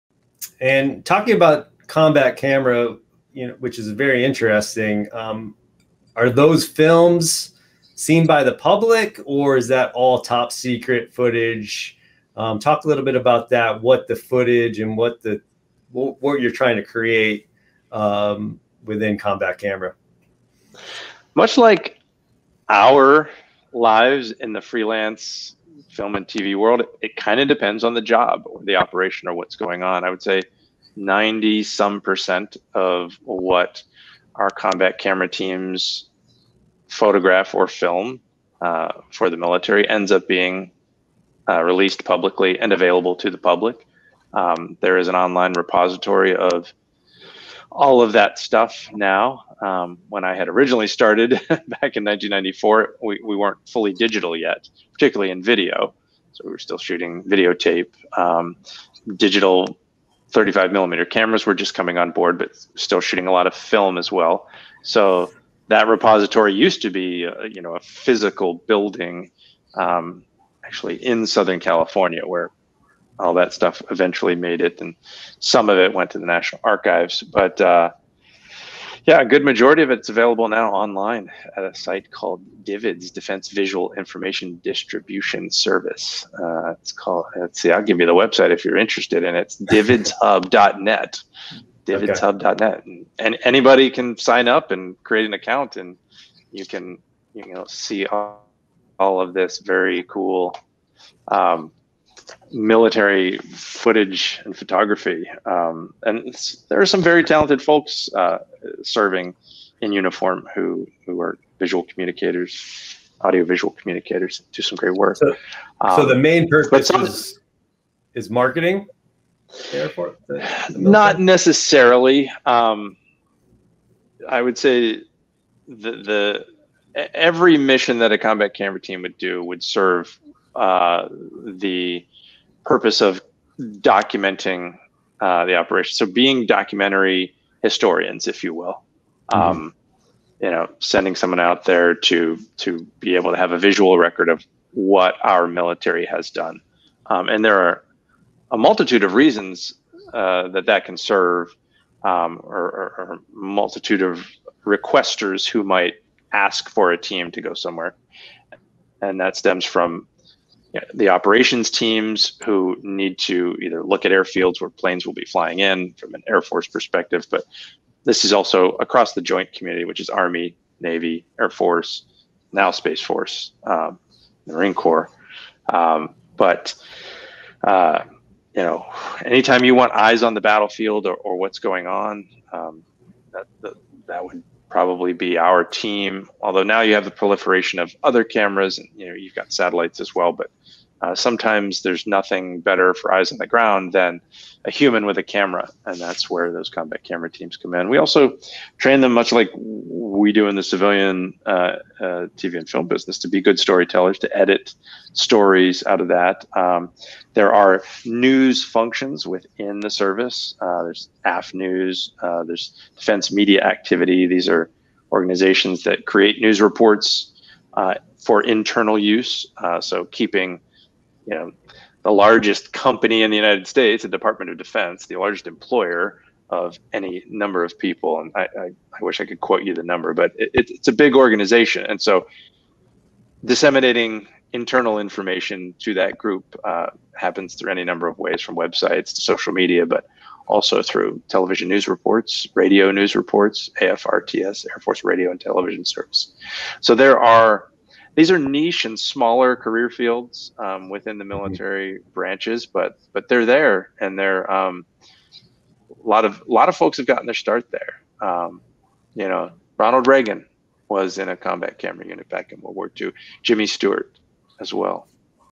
Jim Fabio On The Combat Camera
Jim Fabio, an extraordinary documentarian who has won multiple Emmy awards while serving as a Colonel in the Air Force Reserve overseeing the Combat Camera unit. Here, Fabio explains the use and experience of using combat cameras.

In this conversation, we talk about why Fabio was inspired to attend the Air Force Academy, how a downsizing of the military in the 90s derailed his hopes of becoming a pilot in the Air Force but then led to his award-winning career and we focus the spotlight on his short film: Orangeburg: A Town, A Team, and an American Tragedy

& then listen to the Pod!